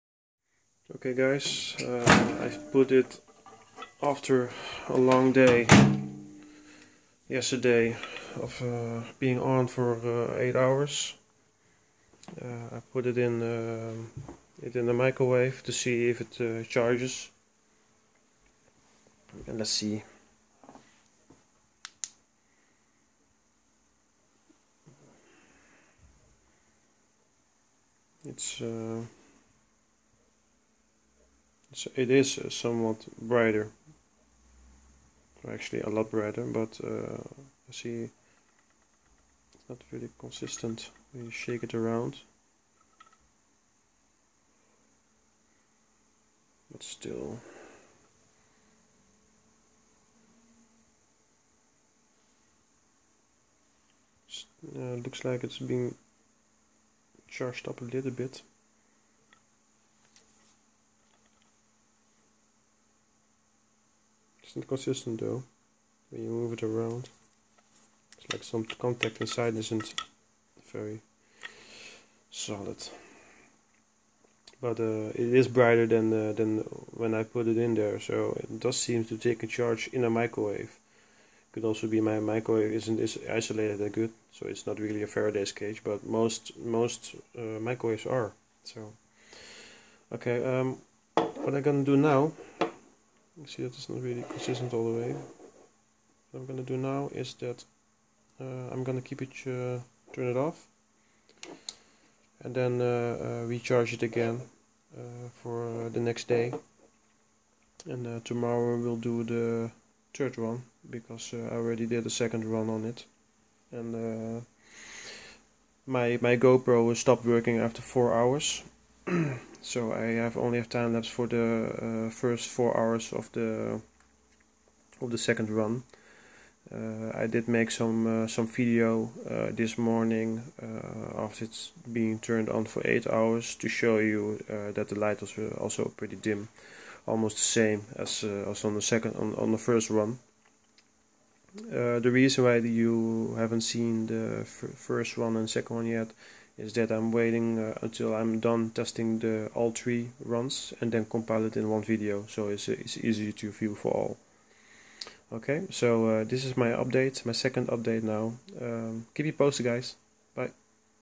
ADGEX - ELFE - Free Energy Flashlight - Update #2
Estimates - will update when all test runs are complete and video has been inspected!

Testrun #1 : runtime 11 hours
Charge time before first update video : 4 hours
Charge time till testrun #2 : 32 hours
Testrun #2 : 8 hours - light was still on but dimmed
Charge time in Microwave (faraday cage) : 4 hours - result light was bright again.

The ELFE flashlight, designed in Australia, by a Russian team, from what I can gather, produces 120 Lumens, and you never have to change the battery, because the "Adgex Accumulator derives its energy from the Earth's magnetic fields, from solar radiation, and from industrial and environmental electromagnetic noise."